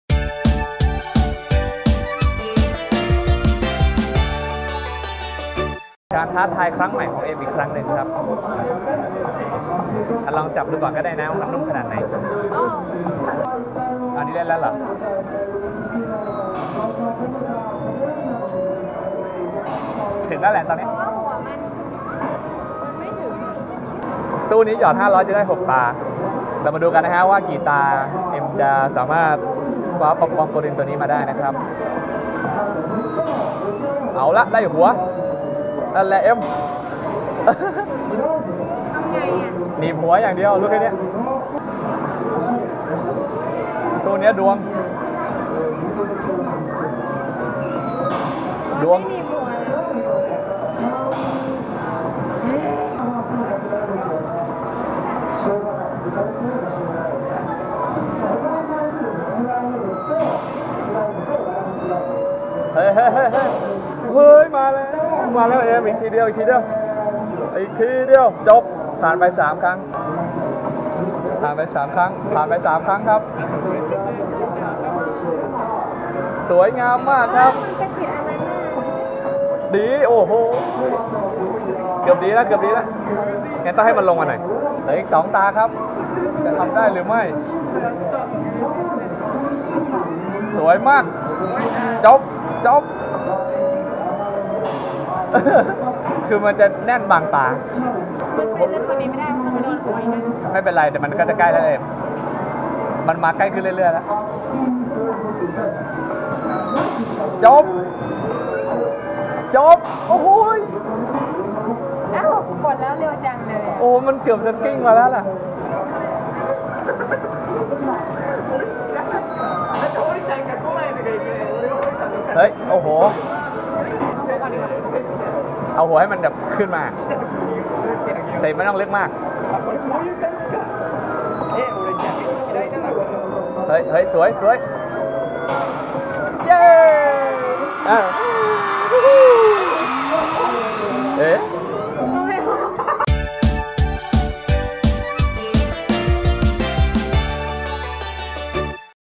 ตู้หนีบตุ๊กตาญี่ปุ่น ร่วงซะปอมปอมปุริน ตอน 5 (UFO Catcher in Japan EP 5)
เชฟเอ็มได้ใจหลังจากคว้าตัวเล็กๆมาได้ตลอด ขอจัดหนักข้ามขั้นเอาตัวใหญ่ไปเลย เพราะปอมปอมปุรินตัวใหญ่ทั้งนุ่มทั้งนิ่มน่ากอด ใครเห็นเป็นต้องเล่น เลยจัดไป แต่มันไม่ง่ายนะจ๊ะไม่รู้ว่าได้หรือเปล่า ต้องมาลุ้นกัน อิอิ

ติดตามช่องทาง Social ต่างๆได้ที่:

** สามารถติดต่อเรื่องโฆษณา สปอนเซอร์ และประชาสัมพันธ์ได้ทาง **